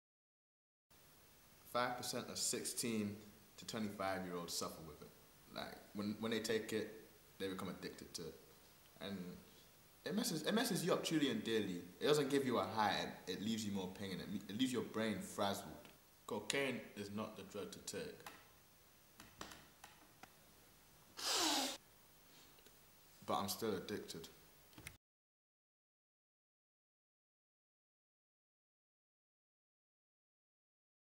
What cocaine does to you | addictive (ORIGINAL VIDEO)
This is a video and a message to those around the world who takes drugs and also this shows how easy you can become addicted to drugs.